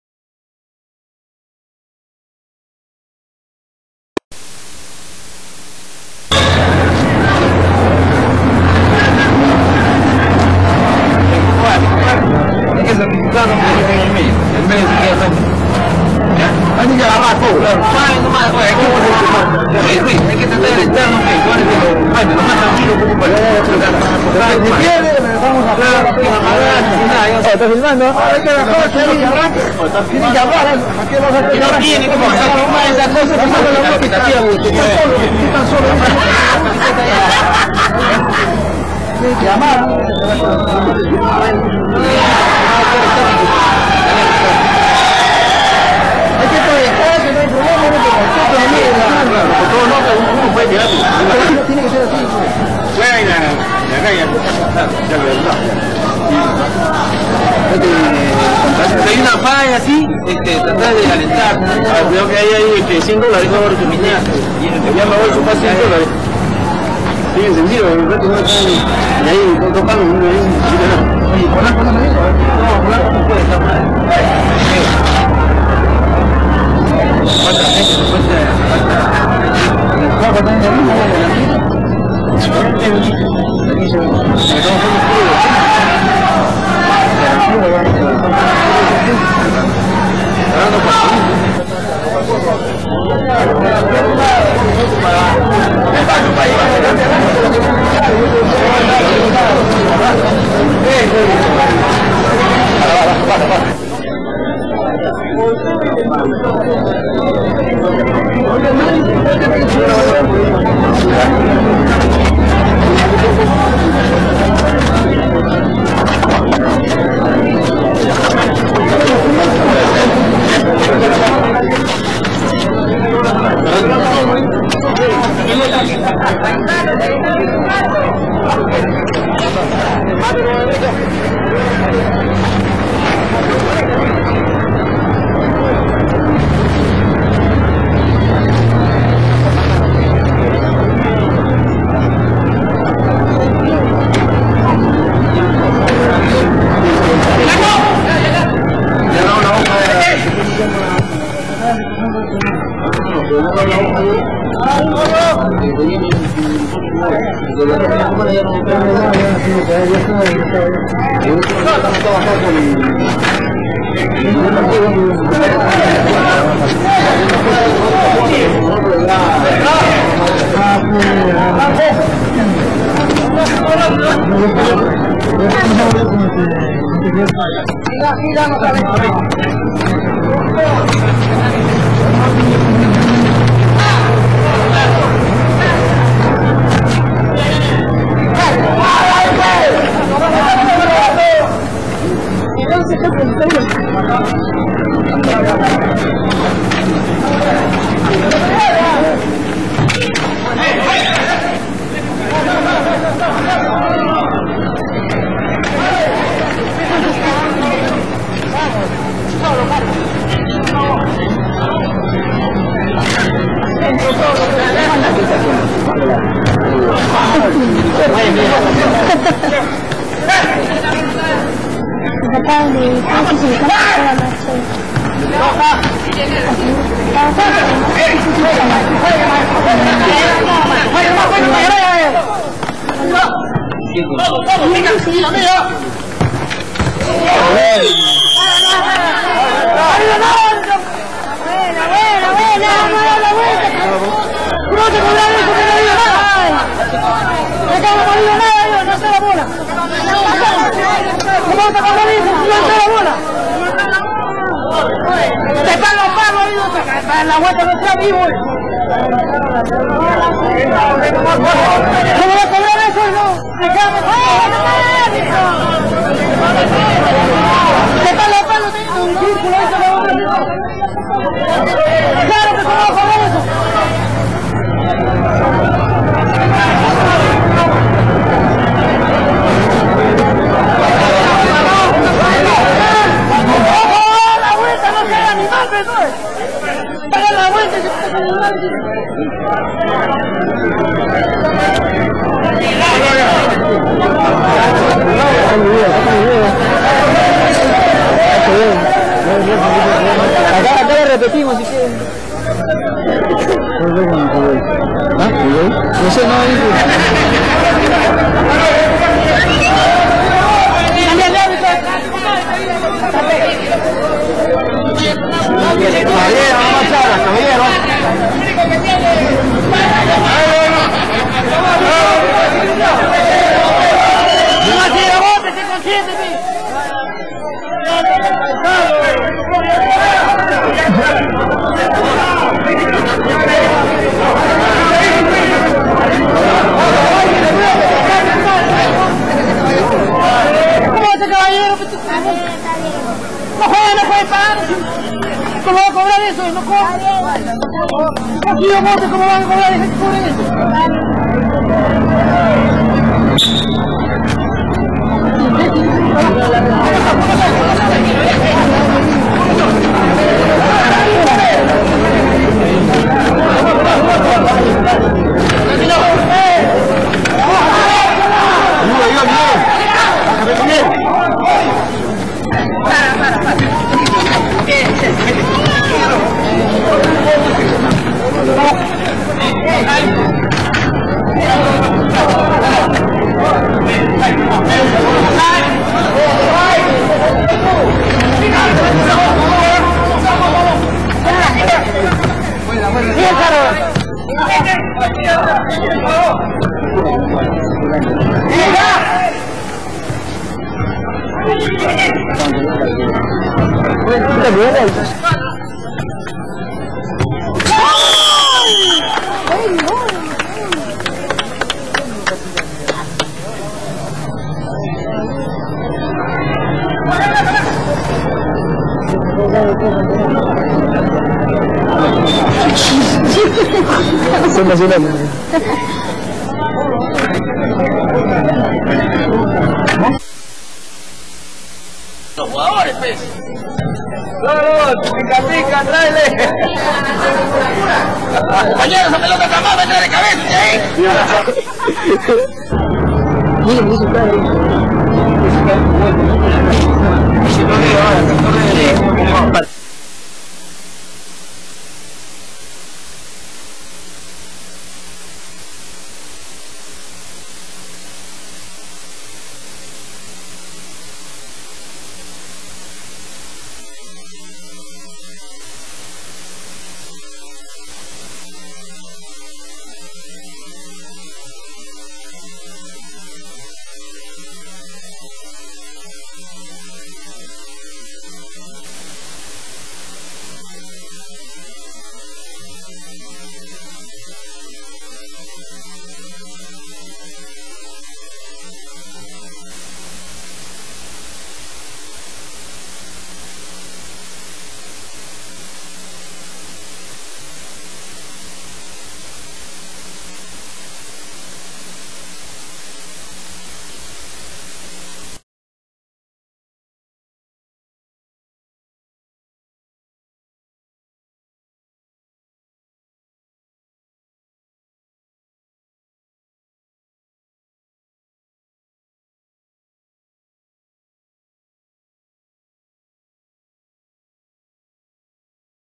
CULTURAL PEDREGAL once estrellas viejas glorias 1993
grabado en 1993 en la hindrandina de moyopampa...viejas glorias que dieron alegria a todo pedregal en los años 80